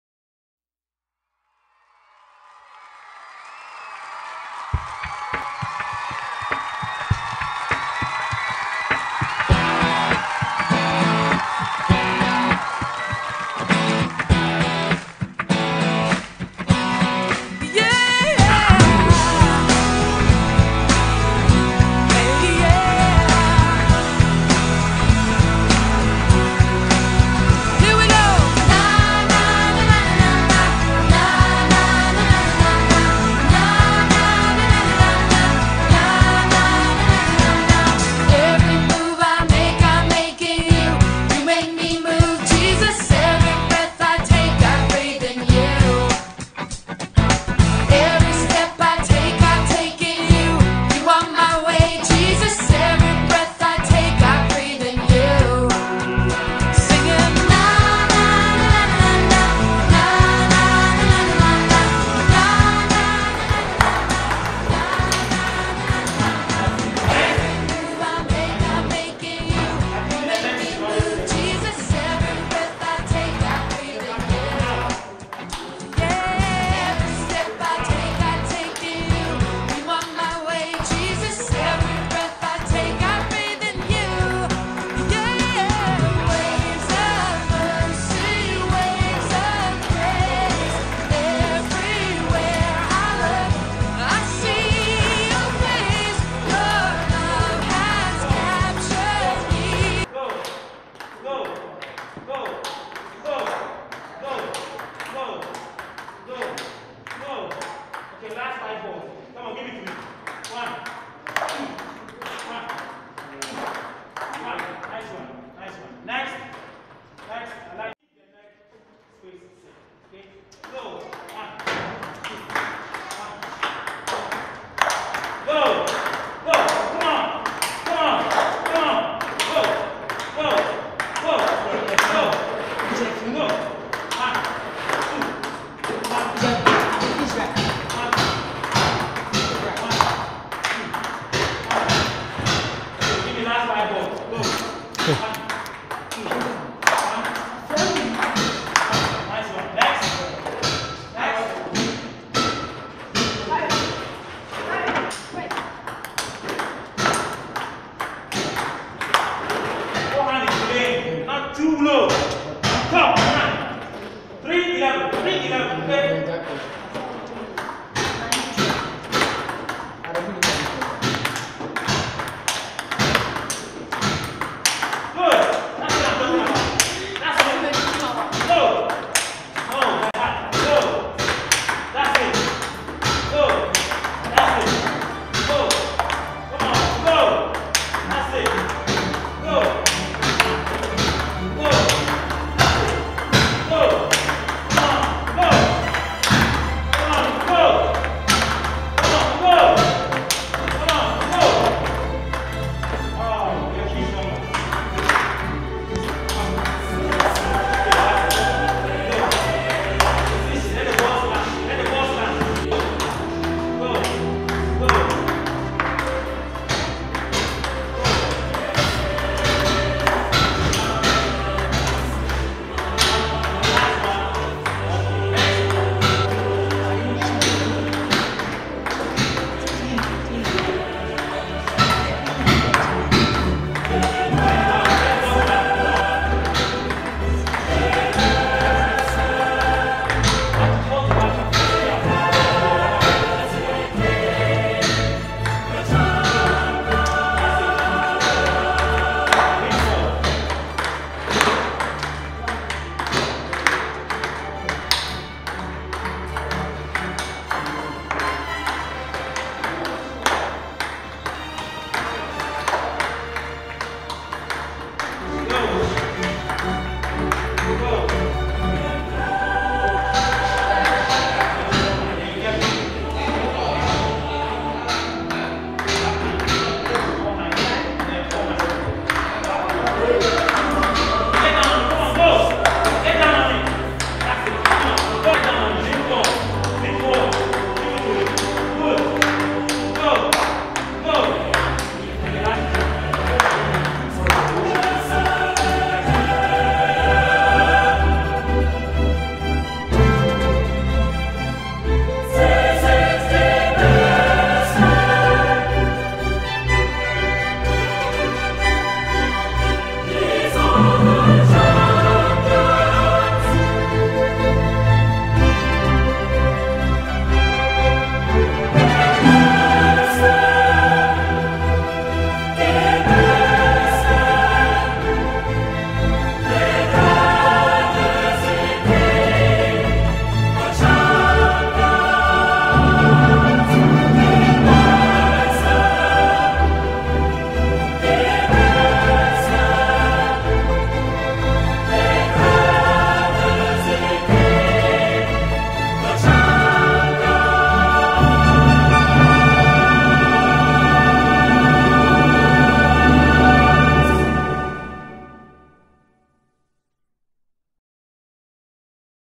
ASHAIMAN TABLE TENNIS CLUB (ATTC) CADET TRAINING SESSION
ATTC which is located in Zenu, is one of the vibrant table tennis clubs in  the Greater Accra region of Ghana. The club was started in 1994 by Richard Golighty who represented Ghana for the 1st African Junior Championship hosted by Egypt in 1986. He won silver and bronze medals in that competition.
For further enquiries the following can be contacted...
RICHARD GOLIGHTLY ...0207836506
BERNARD JOE SAM .0243926781
DAVID SENYO .0243505396